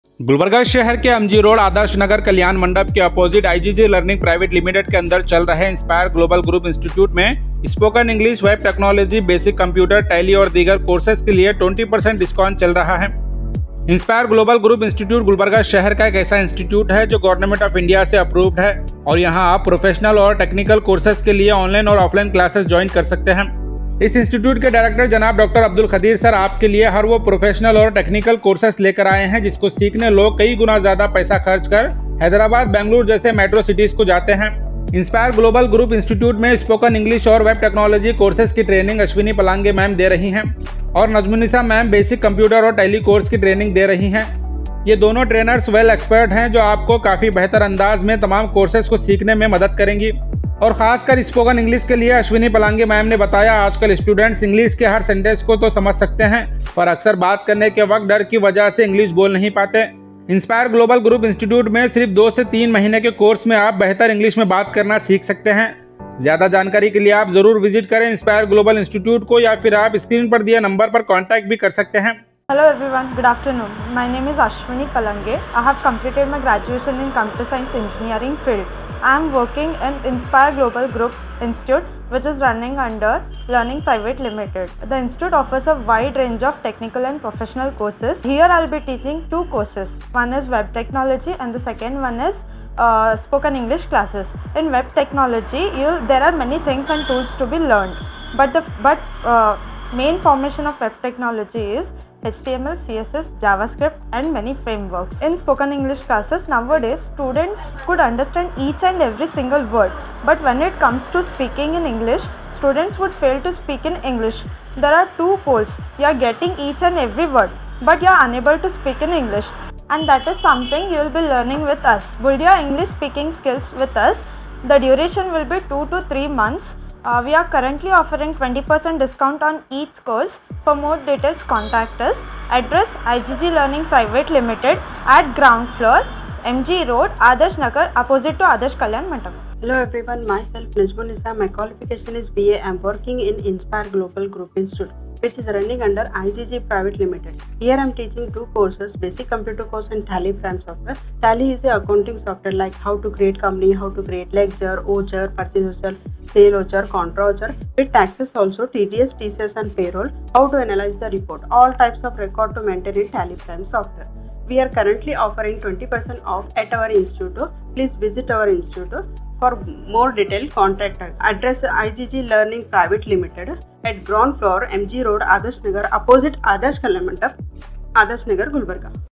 Inspire Global Group Institute is offering 20% discount on many courses, Visit MG Road, Gulbarga...
Gulbarga Inspire Global Group Institute is offering 20% discount on courses such as Spoken English, Web Technology, Basic Computer, Tally, and others. Visit MG Road Opp.